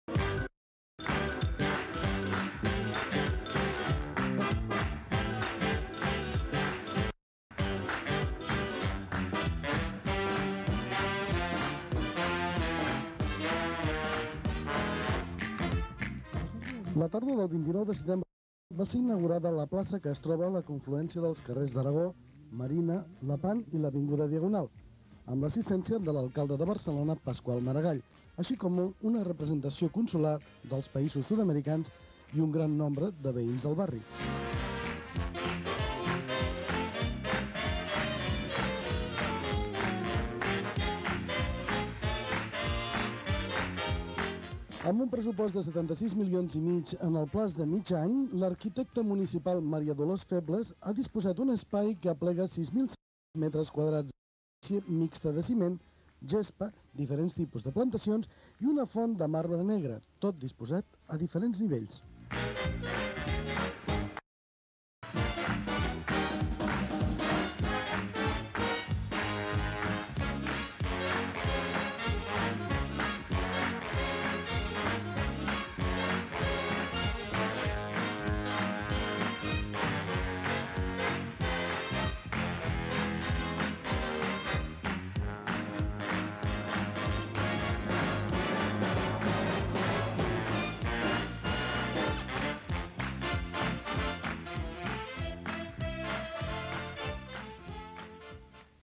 1989 10 05 Inauguracio Plaça Arago Marina Diagonal
El dia 1 d'octubre va tenir lloc l'Inauguració de la plaça de la Hispanitat, situada a la cruïlla dels carrers Aragó, Marina, Lepant i Avgda. Diagonal.